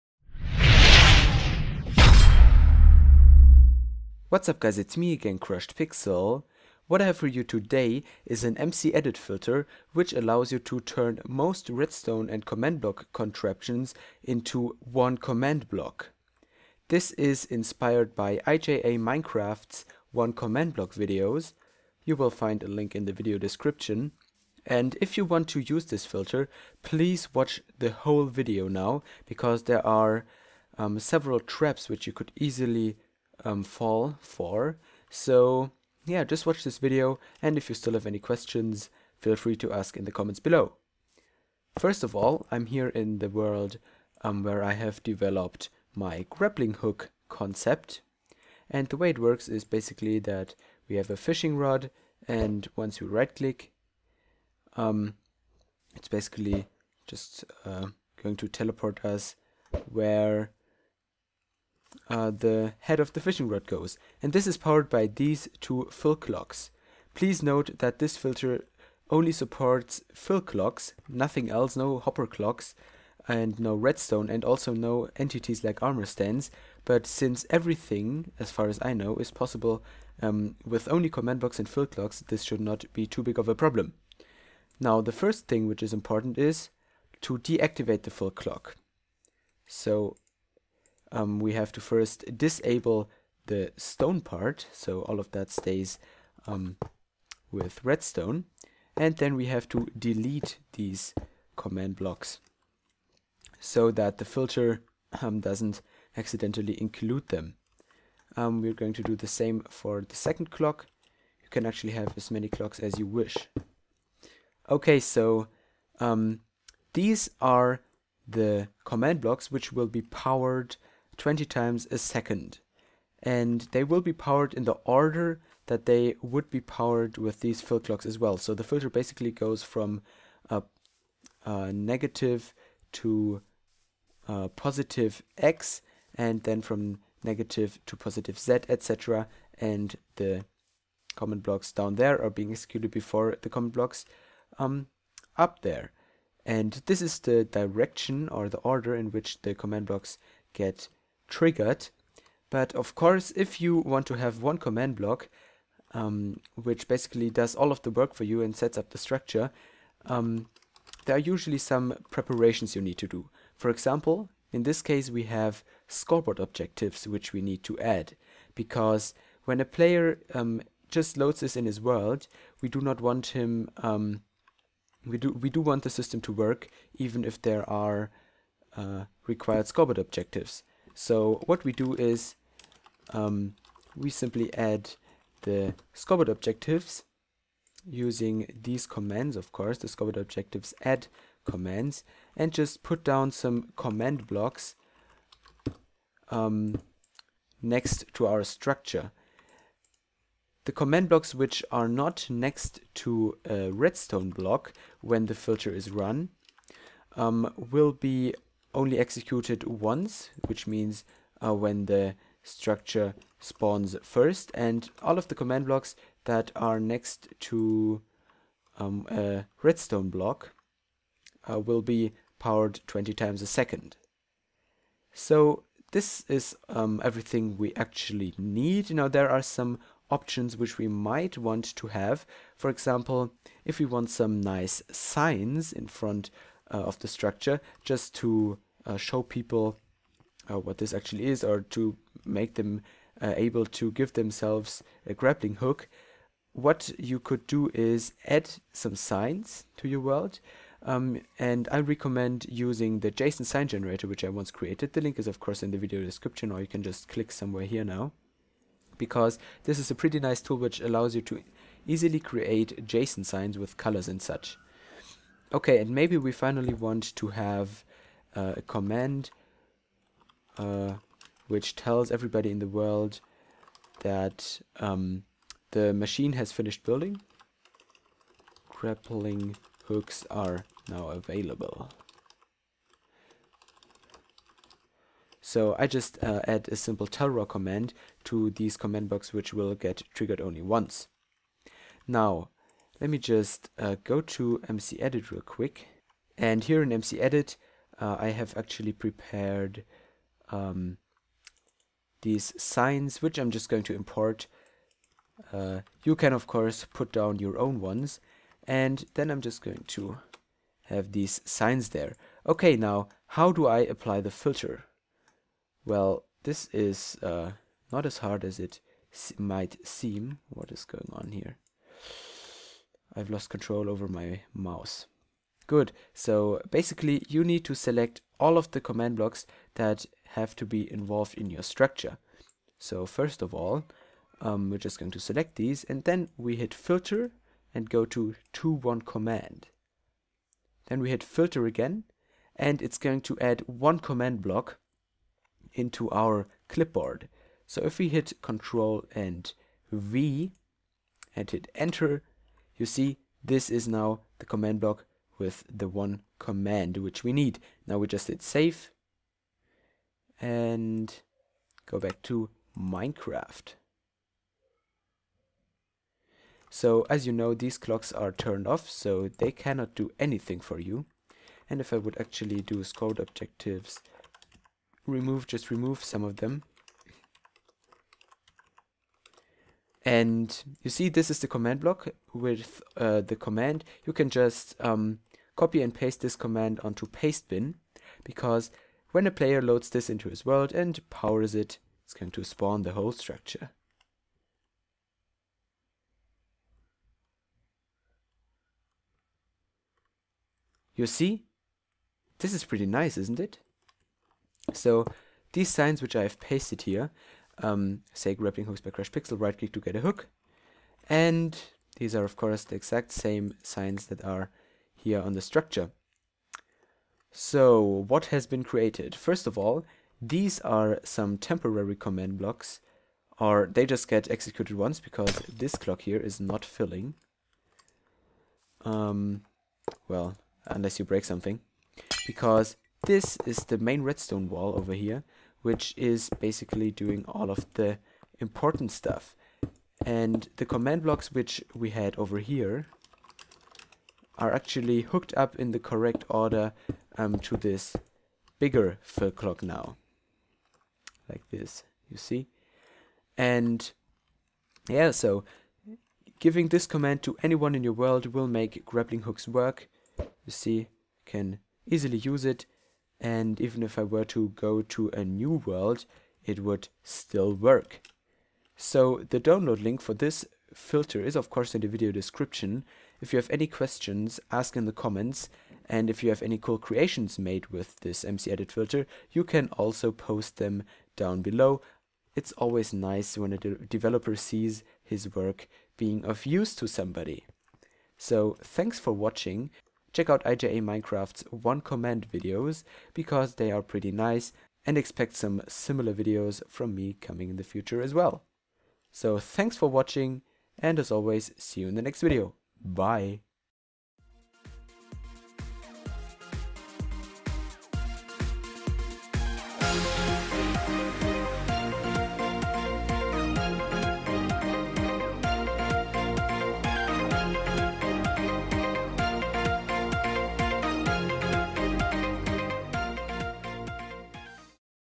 To One Command MCEdit Filter | Command Block Creations in one Command!
I have created an MCEdit Filter to automatically generate a command that spawns an entire Command Block Contraption!

As always, leaving a like makes me happy :)

Server IP: crushedpixel.eu
TeamSpeak IP: ts.crushedpixel.eu